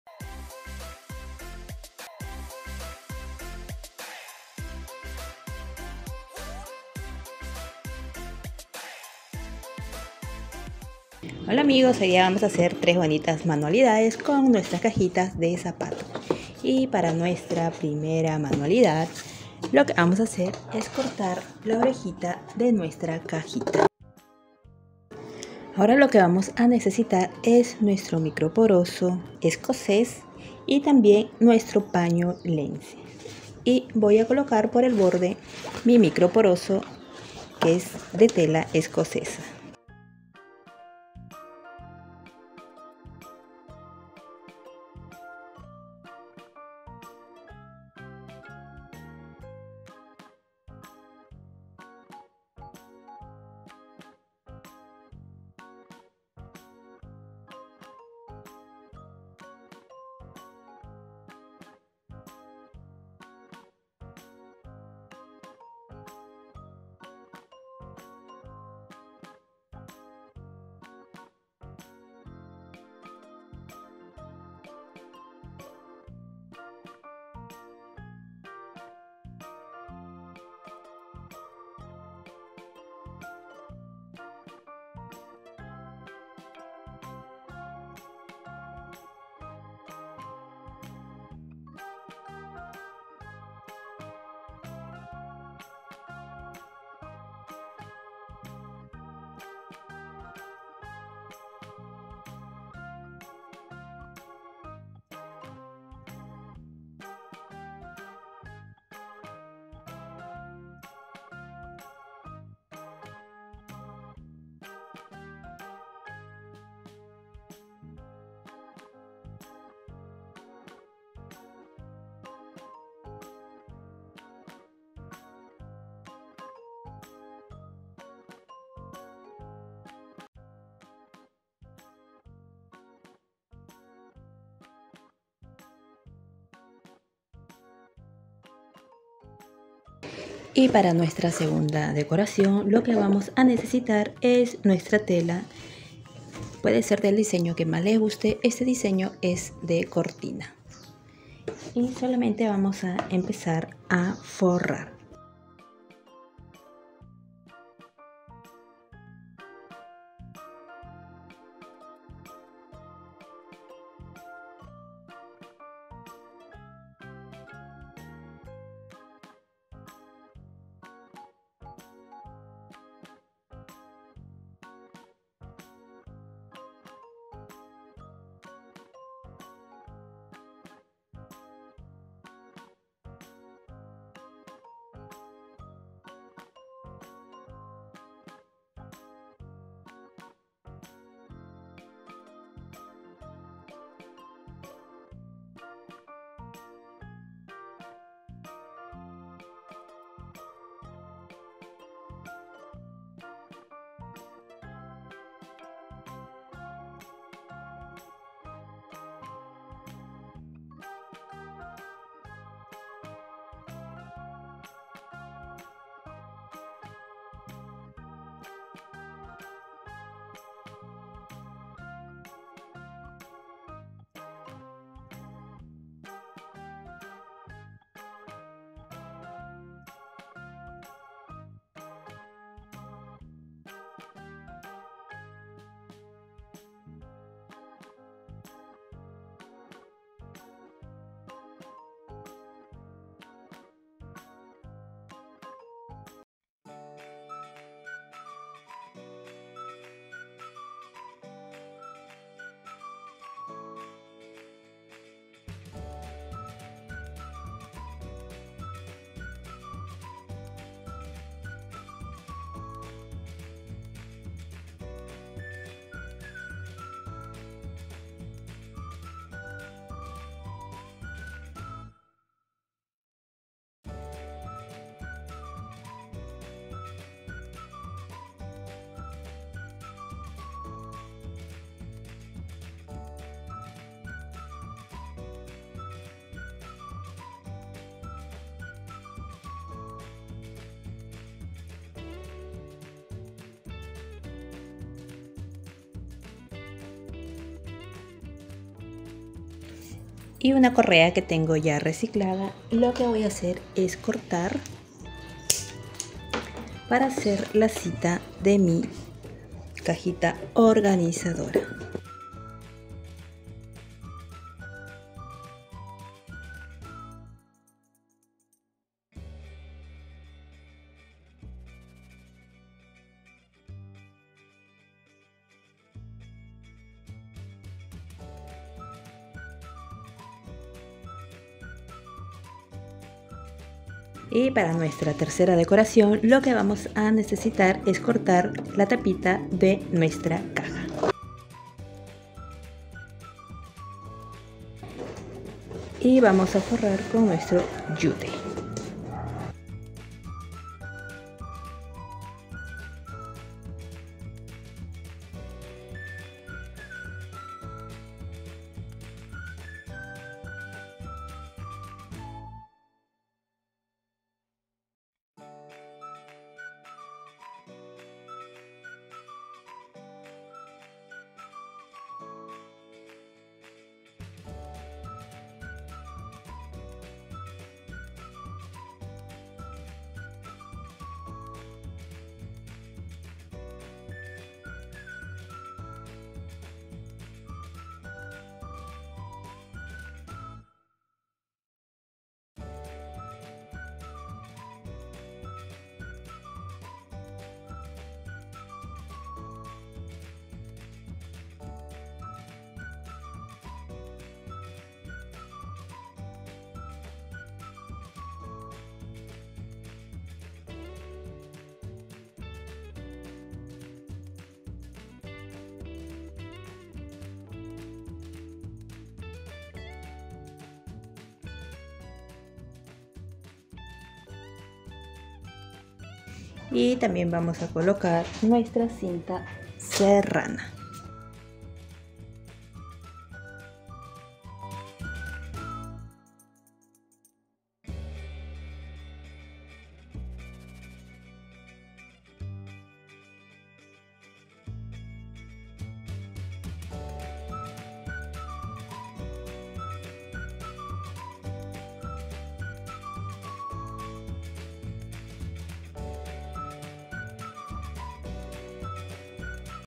¡POR QUE COMPRAR ORGANIZADORES DE TIENDA SUPER CARO!! SI LO PUEDES HACERLO TÚ
Hola Amig@s 🥰 Tienen cajas de Zapato no los tires!! vamos hacer hacer Hermoso Organizadores como de tienda 🫣 Acompáñenme a ver el video & Disfrútenlo. 😍   💕 &

✅ MATERIALES 👉
_ CAJAS DE ZAPATO
_ TELA DE CORTINA Ó LAS QUE LES GUSTE
_ PAÑO LENCY
_ MICROPOROSO TELA ESCOCES
_ TELA YUTE
_ CINTA SERRANA
_ TIJERA